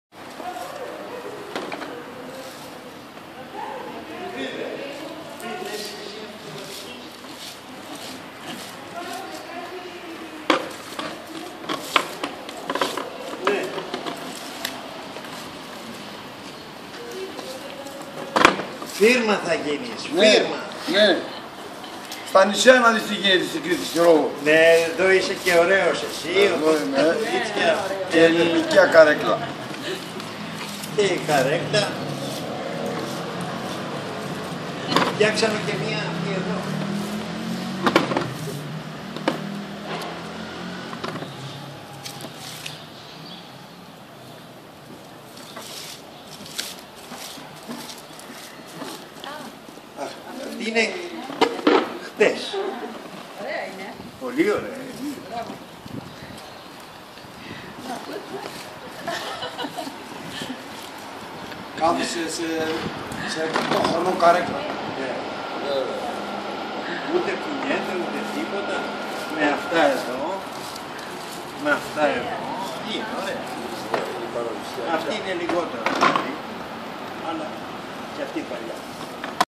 Greek chairs
How you make or repair Greek chairs? It looks simple, but it needs experience! Video taken in centre Athens, Irakleidon street, close to the Acropolis.  If you want your chair to get the Greek flavor, look for him in this street!